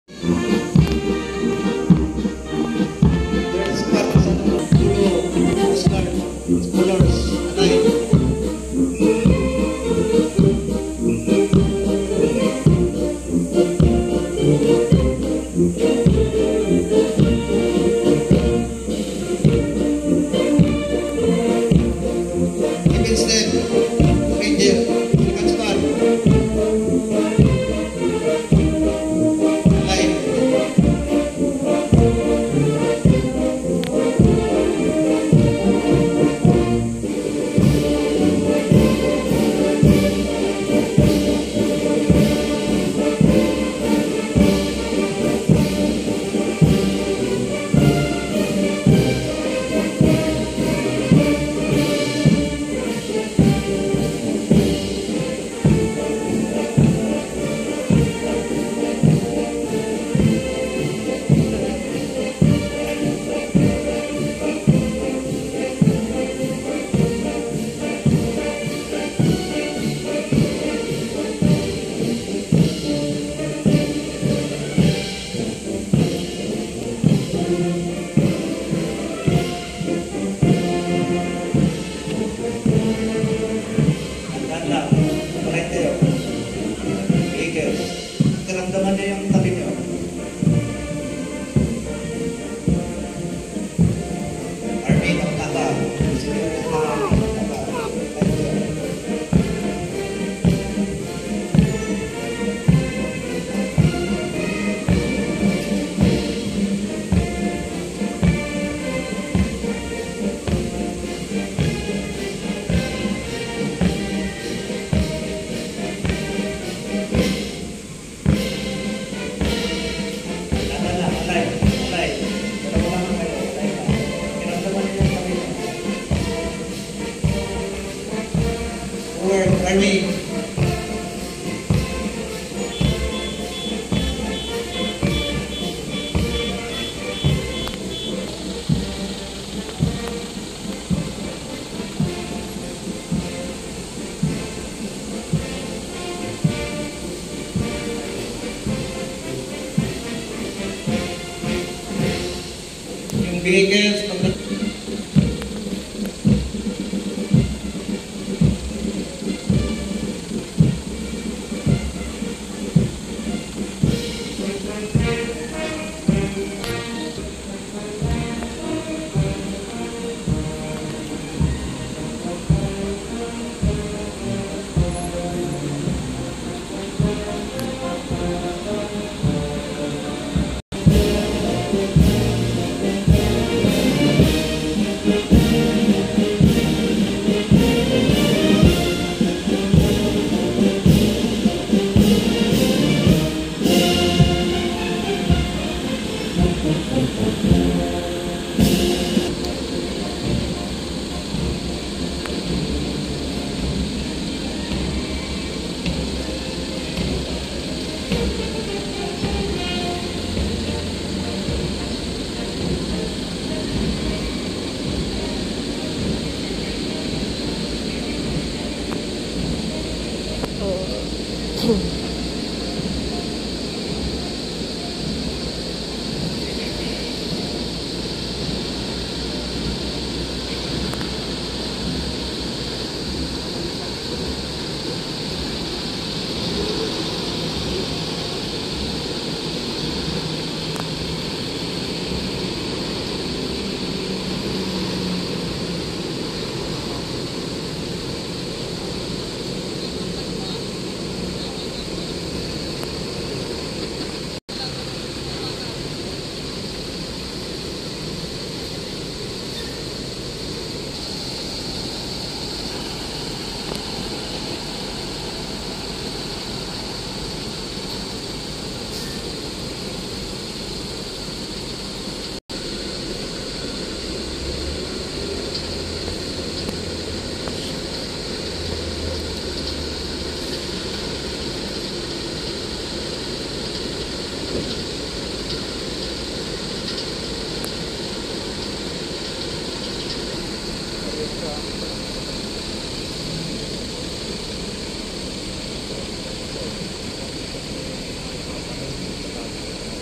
PHILIPPINES FULL MILITARY FORCE
Rehearsal for the Inauguration/Oath-taking Ceremony of President-elect Ferdinand Romualdez Marcos Jr. at Lapu-lapu Grandstand Camp Aguinaldo